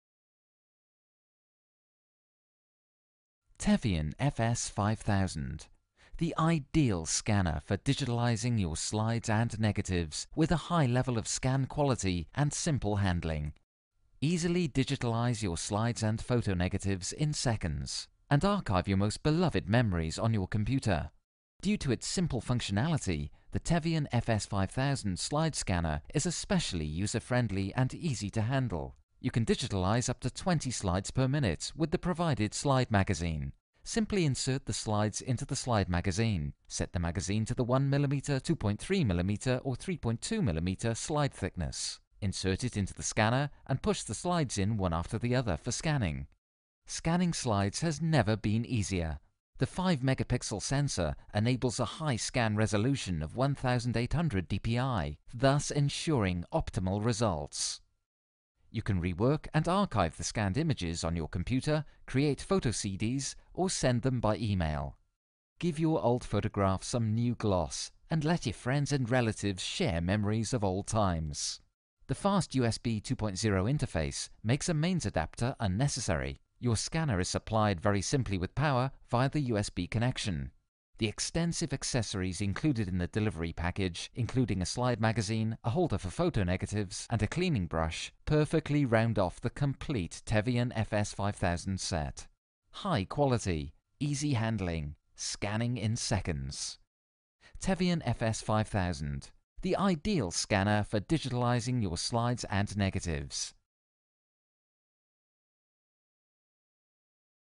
Tevion FS 5000 scanner Nimofilm download link in description
Key, if you need it:  m133de24edaf77d9   Extract the .zip, enter the nimofilm folder and run the nimofilm.exe